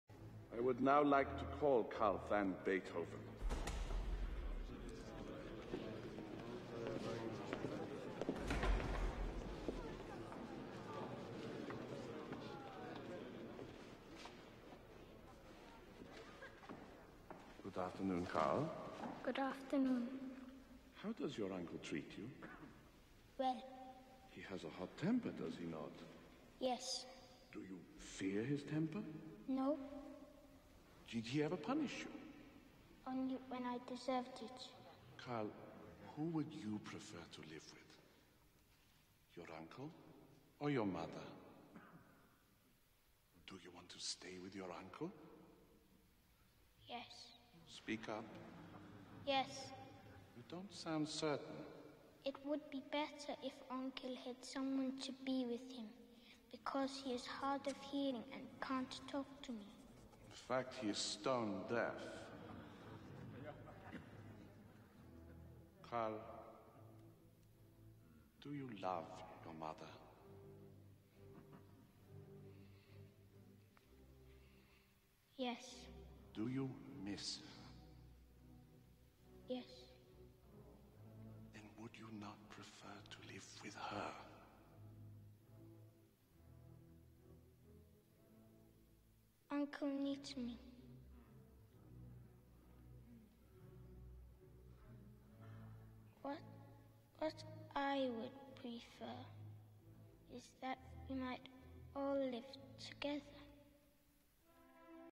The legal custody Battle
In 1815, his brother Caspar died, leaving a nine-year-old son, Karl, to whom Beethoven and Caspar's widow became co-guardians. Young Karl was the object of a savage tug-of-war. For five years, Beethoven fought legal battles for exclusive custody of his nephew; he finally won.